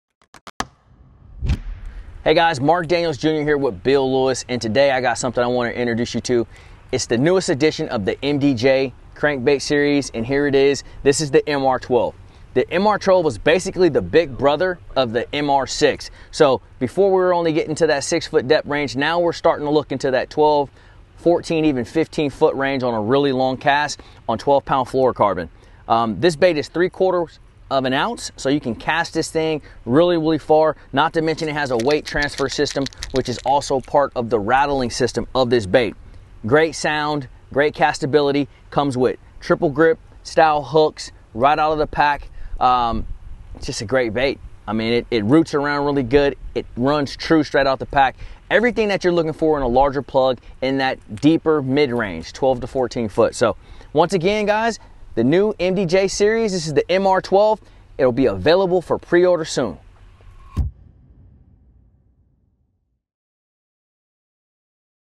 Bill Lewis MR-12 Crankbaits with Mark Daniels Jr | First Look 2021
TW discusses the New Bill Lewis MR-12 Crankbaits with Mark Daniels Jr as part of Tackle Warehouse's First Look coverage - an in-depth look at the newest fishing products debuted in Summer 2021.